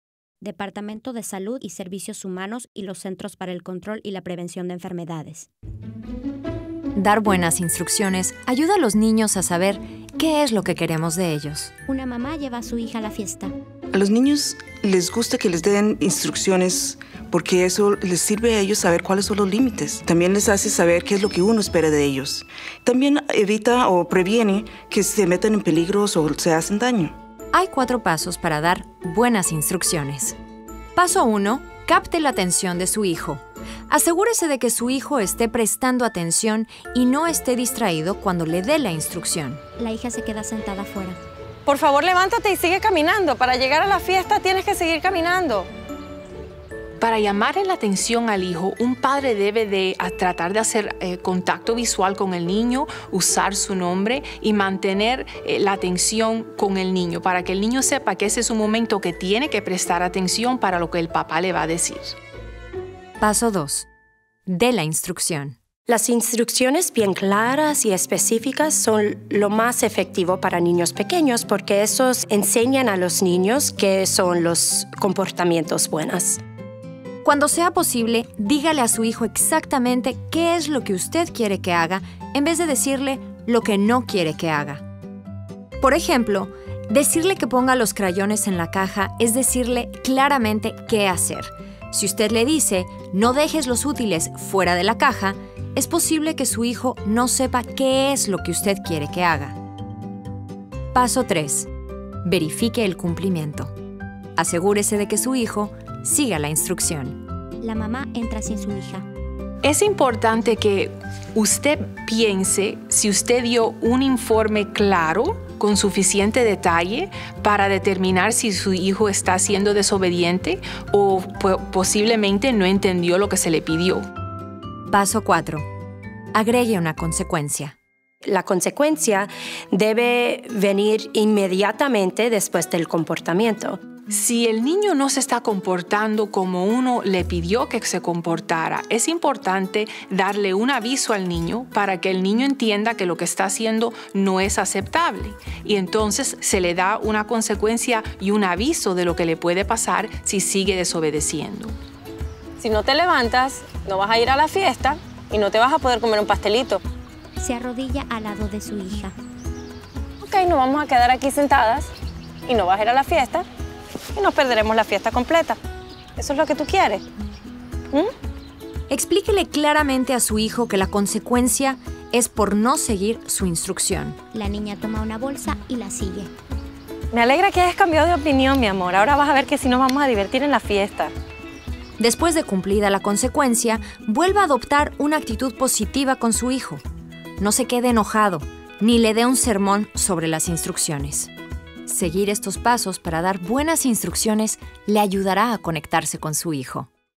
Dar instrucciones con la descripción de audio
Aprenda los pasos para dar instrucciones y descubra qué hacer cuando su hijo no le hace caso.